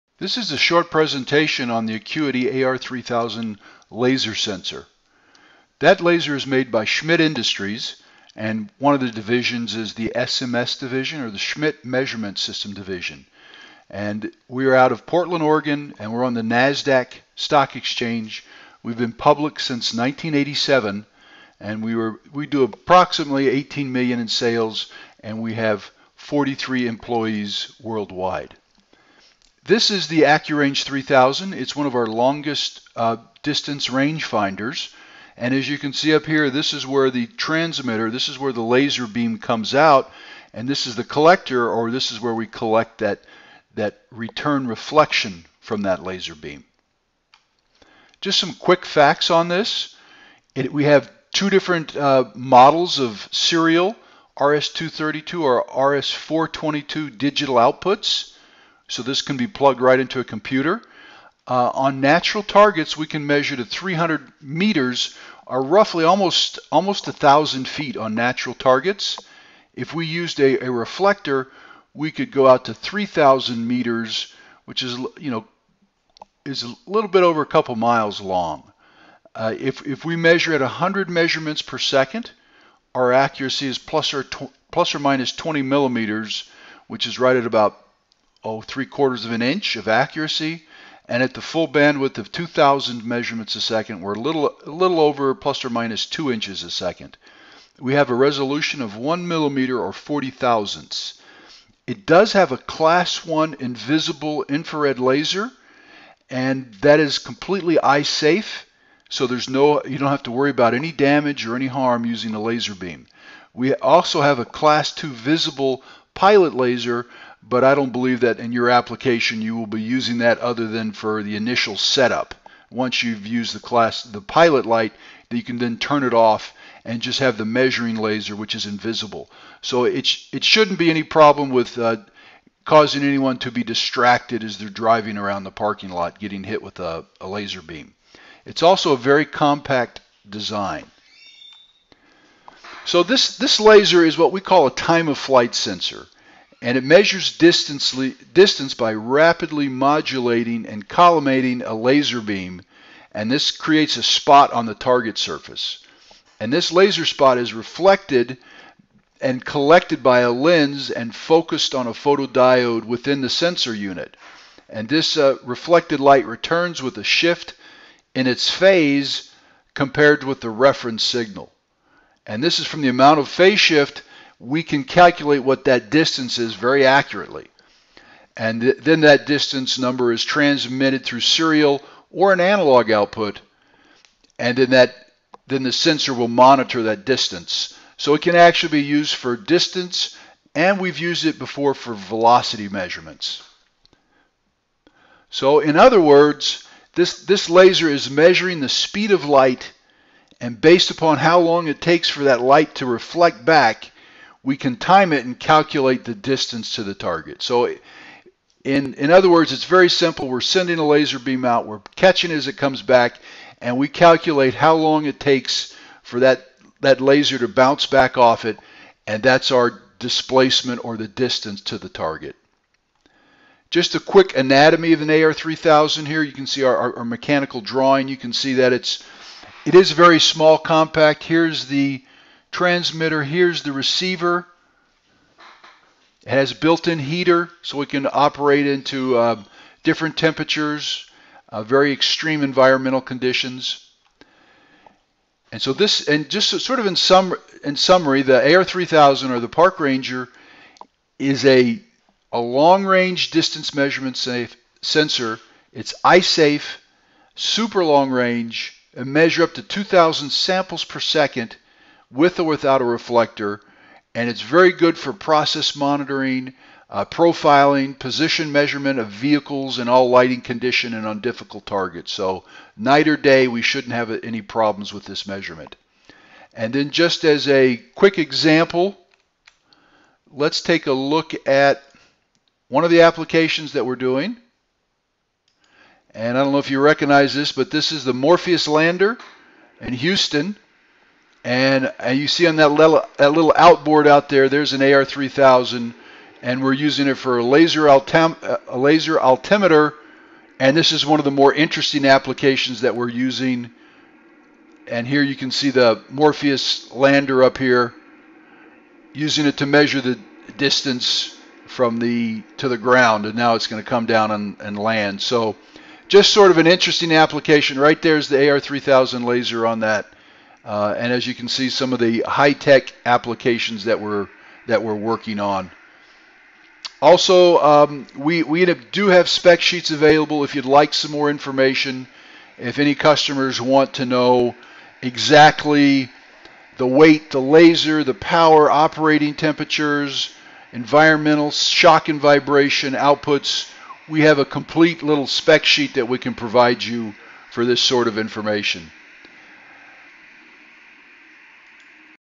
Time-of-Flight Laser Sensor Overview
This is a short overview on a time-of-flight laser sensor by using our Acuity AR3000 laser sensor as an example. The Accurange 3000 is one of our longest distance rangefinders.